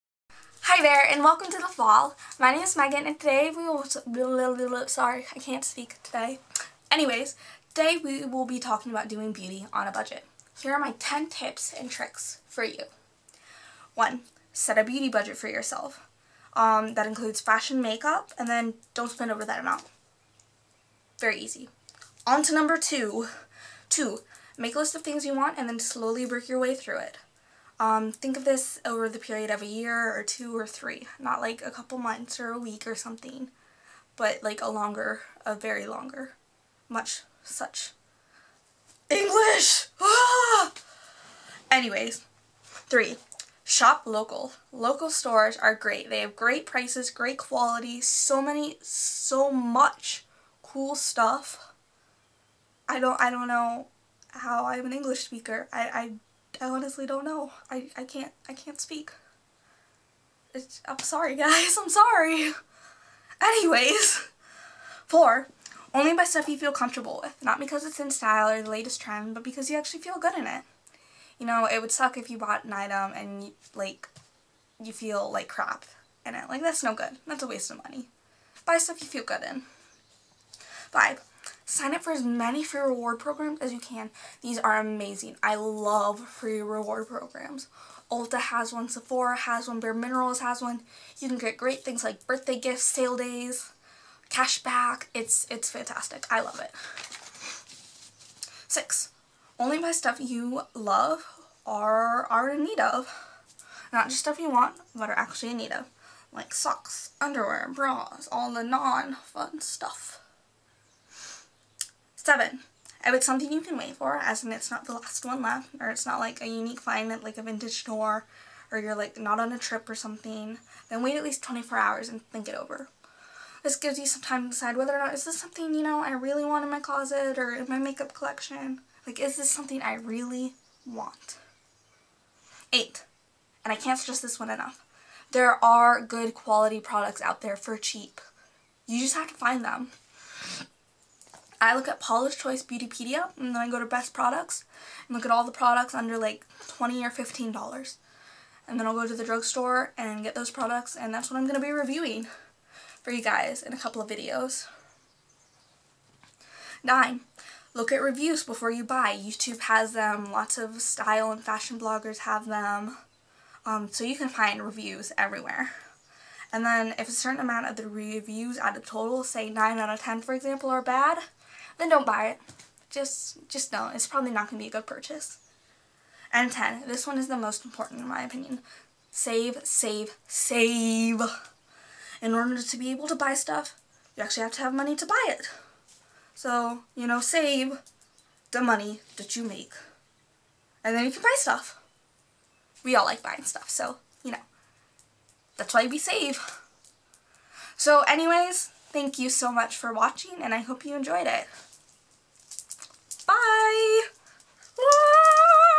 10 Tips for Beauty on a Budget
Feel like you're falling down the rabbit hole into beauty wonderland? Keep calm and learn about fashion and makeup while on a budget! Discover how to keep yourself from falling into the trap of spending excess amounts of money while learning the ins and outs of fashion and makeup. This channel will feature product reviews, hauls, interviews, and more!